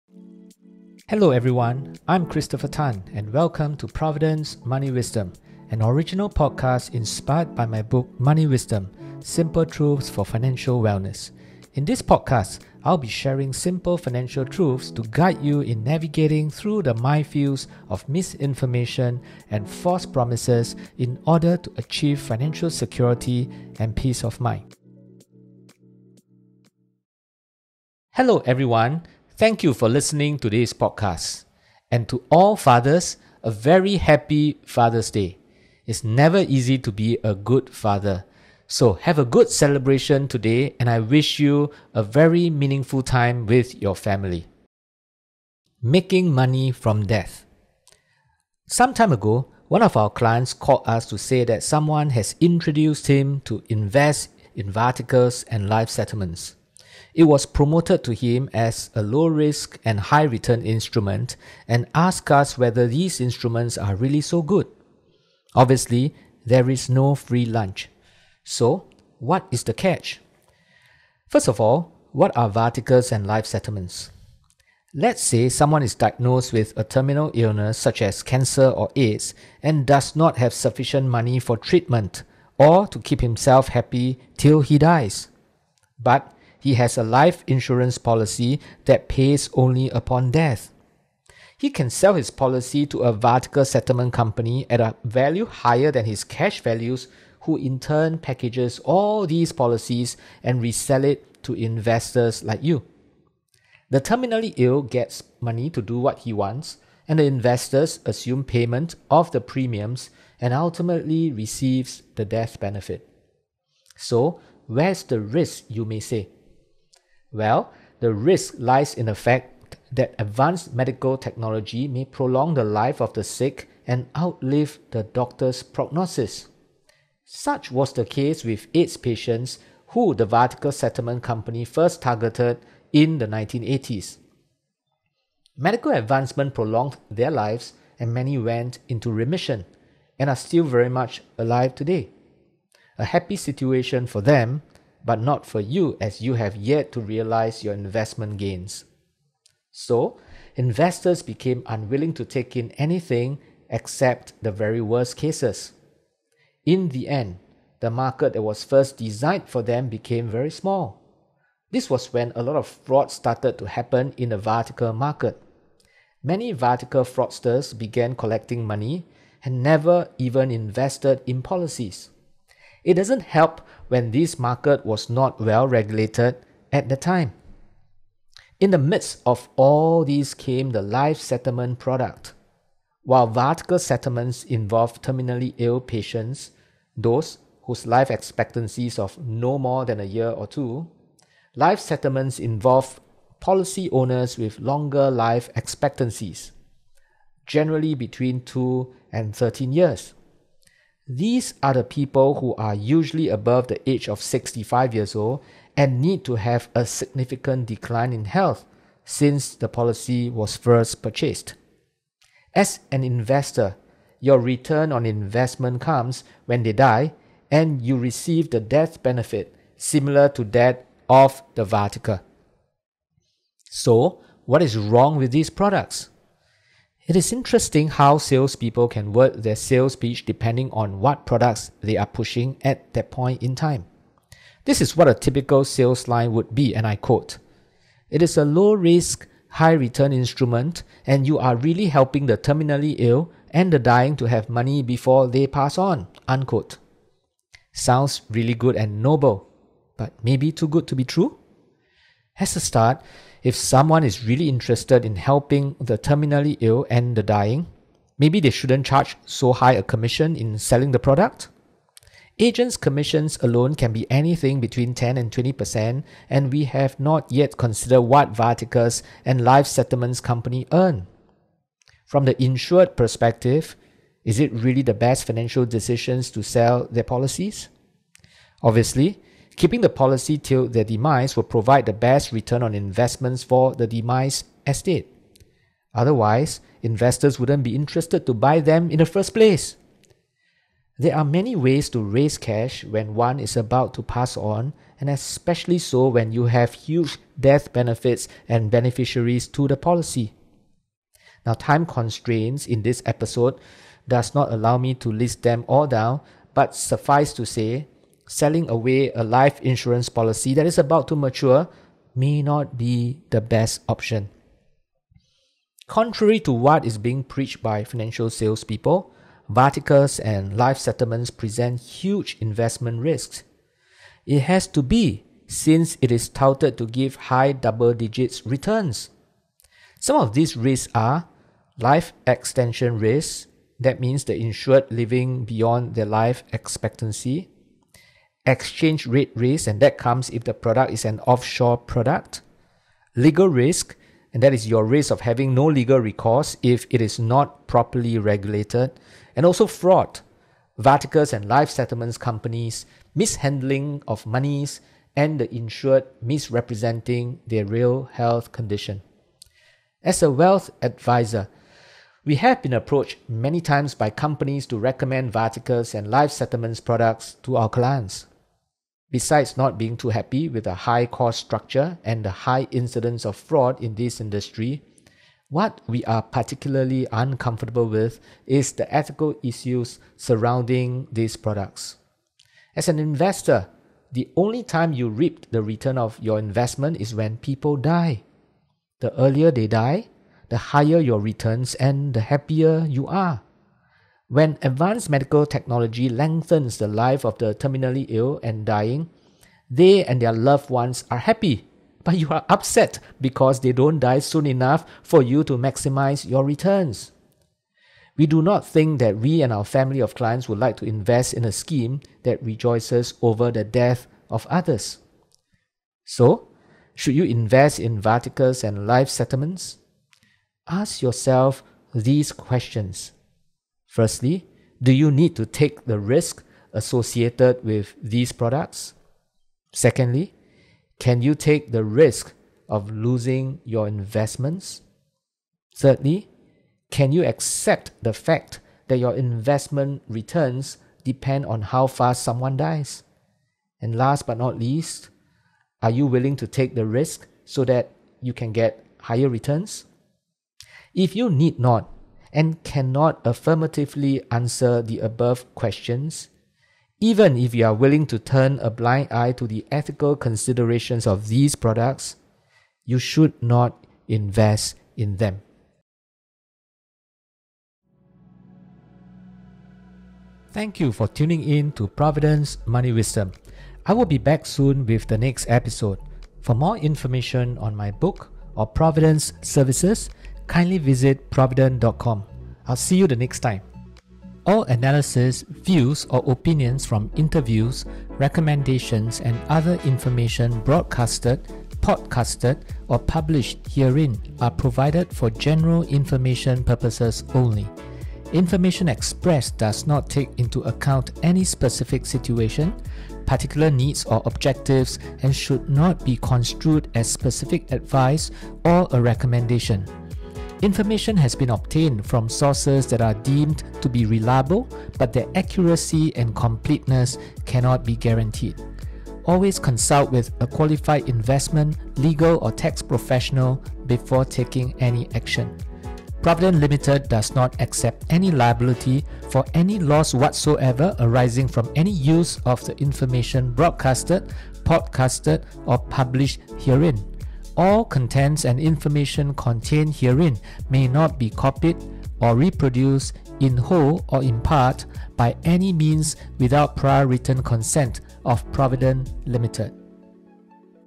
Episode 22: Making Money from Death
Can you accept the fact that your investment returns depend on how fast someone dies? The earlier they die, the higher your returns.

In this episode, I will share my professional insights on viatical and life settlements, explaining the problem with these products in terms of ethical considerations and the risks involved.

Important note: There are many ways to make money, and nevertheless, there are some monies you should not make.